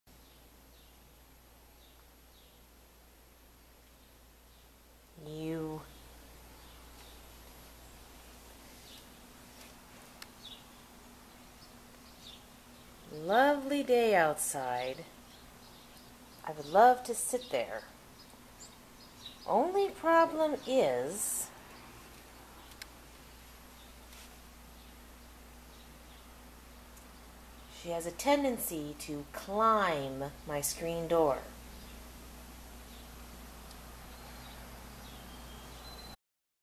Springtime fun... precluded
It's only happened once - problem is, I was on the other side of the screen and she got herself good and stuck.